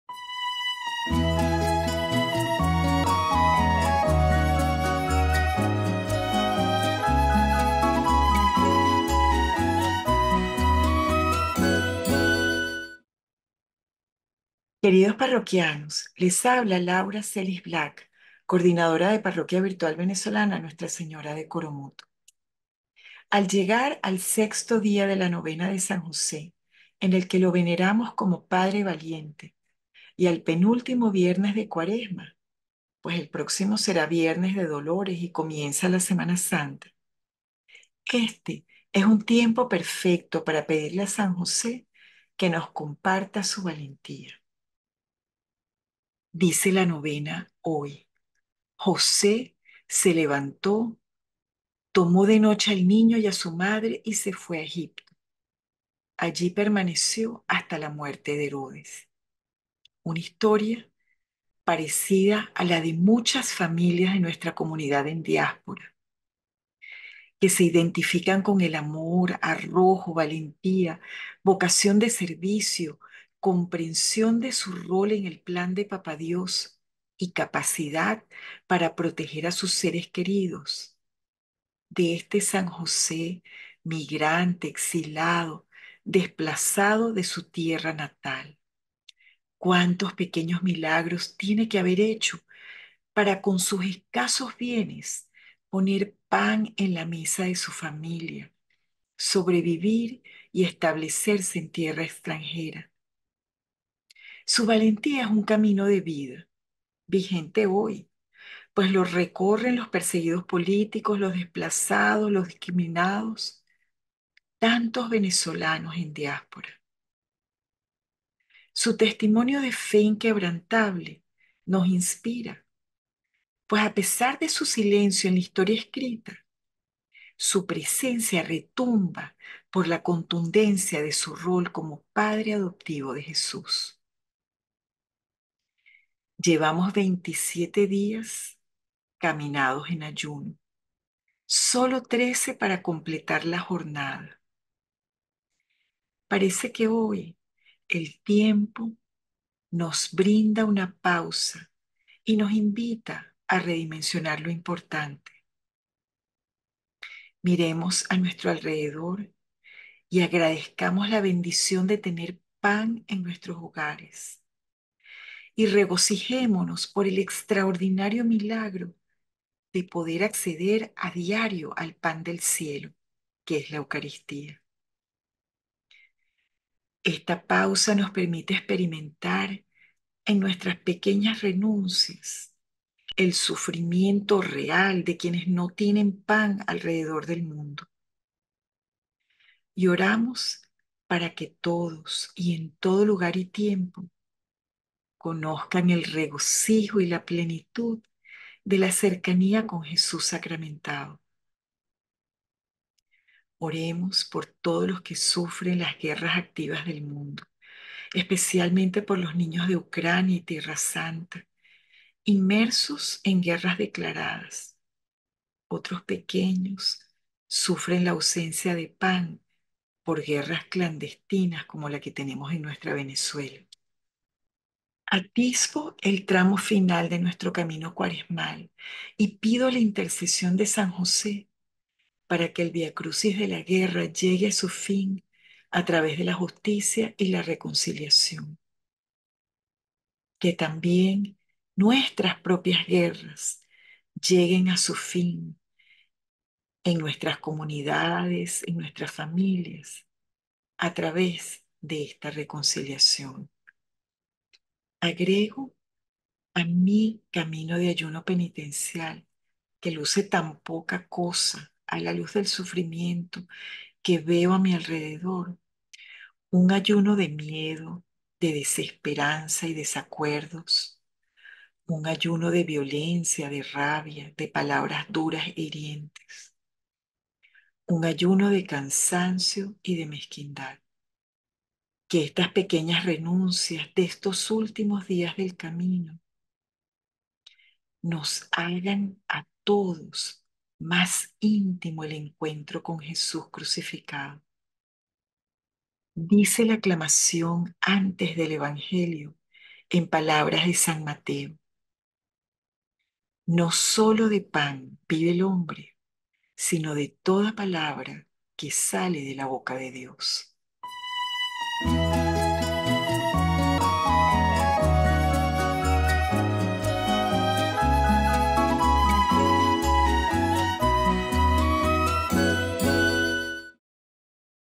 DIA 27 CAMINO CUARESMAL Viernes de la Cuarta Semana de Cuaresma Marzo 15, 2024 Palabra PAN
Día 27:
CAMINO CUARESMAL
Viernes 15 de Marzo, 2023
Palabra: PAN

Queridos parroquianos:
Les habla Laura Celis Black, coordinadora de Parroquia Virtual Venezolana Nuestra Señora de Coromoto.

Al llegar al Sexto Dia de la Novena de San José, en el que lo veneramos como Padre Valiente, y al penúltimo viernes de Cuaresma, pues el próximo será viernes de Dolores y comienza la Semana Santa. Hoy es un TIEMPO perfecto para pedirle a San José, que nos comparta su valentía. “José se levantó, tomó de noche al niño y a su madre, y se fue a Egipto. Allí permaneció hasta la muerte de Herodes” (Mt 2, 14-15). Una historia parecida a la de muchas familias de nuestra comunidad en diáspora, que se identifican con el AMOR, arrojo, valentía, vocación de SERVICIO, COMPRENSION de su rol en el plan de Papá Dios, y capacidad para proteger a sus seres queridos, de este San José, migrante, exilado, desplazado de su tierra natal. Cuantos pequeños milagros tiene que haber hecho para, con sus escasos BIENES, poner PAN en la mesa de su familia, sobrevivir y establecerse en tierra extranjera. Su valentía es un CAMINO de vida, vigente hoy, pues lo recorren los perseguidos políticos, los desplazados, los discriminados, tantos venezolanos en diaspora... Su TESTIMONIO de Fe inquebrantable nos inspira, pues a pesar de su silencio en la historia escrita, su presencia retumba por la contundencia de su rol como padre adoptivo de Jesús.
Llvamos veintisiete días caminados en ayuno, sólo trece para completar la jornada, parece que hoy el TIEMPO nos brinda una pausa y nos invita a redimensionar lo importante. MIREMOS a nuestro alrededor y agradezcamos la bendición de tener PAN en nuestros hogares, y nos REGOCIJEMONOS por el extraordinario milagro de poder acceder a diario al PAN del cielo que es la EUCARISTIA. Esta pausa nos permite experimentar en nuestras pequeñas RENUNCIAS el sufrimiento real de quienes no tienen PAN alrededor del mundo, y ORAMOS para que todos y en todo lugar y TIEMPO conozcan el REGOCIJO y la PLENITUD de la CERCANIA con Jesús Sacramentado. OREMOS por todos los que sufren las guerras activas del mundo, especialmente por los niños de Ucrania y Tierra Santa, inmersos en guerras declaradas, otros pequeños sufren la ausencia de PAN, por guerras clandestinas como la que tenemos en nuestra Venezuela.
Atisbo el tramo final de nuestro Camino Cuaresmal, y pido la intercesión de San José para que el Viacrucis de la guerra llegue a su fin a través de la justicia y la RECONCILIACIÓN, que también nuestras propias guerras lleguen a su fin, en nuestras comunidades, en nuestras propias familias a través de esta RECONCILIACIÓN. Agrego a mi CAMINO de ayuno penitencial, que luce tan poca cosa a la LUZ del sufrimiento que veo a mi alrededor, un ayuno de miedo, de desesperanza y desacuerdos, un ayuno de violencia, de rabia, de palabras duras e hirientes, un ayuno de cansancio y de mezquindad. Que estas pequeñas RENUNCIAS de estos últimos días del CAMINO, nos hagan a todos más INTIMO el encuentro con Jesús Crucificado. Dice la aclamación antes del evangelio:
“No sólo de PAN vive el hombre, sino de toda palabra que sale de la boca de Dios.”
(Mt 4,4)